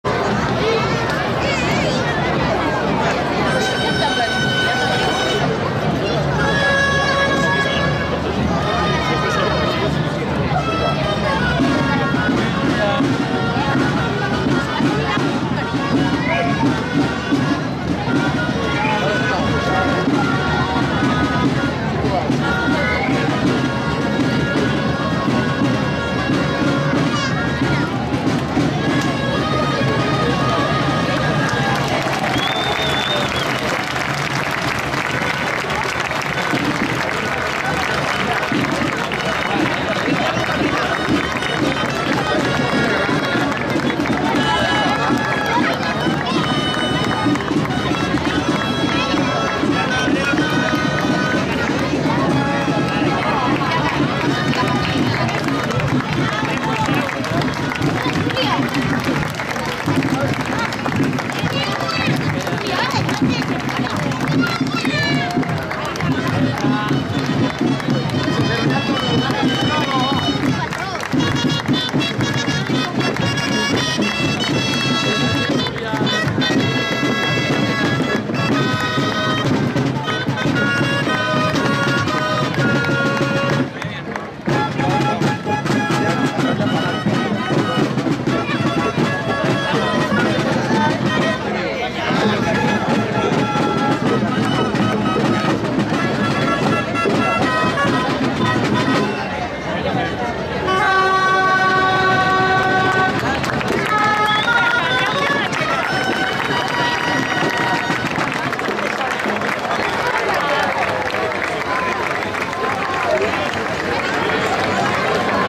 Capgrossos Pilar de 4 caminant, La Passada 2016
27 de juliol, Festa Major de Mataró, Carrer Argentona / Muralla del Tigre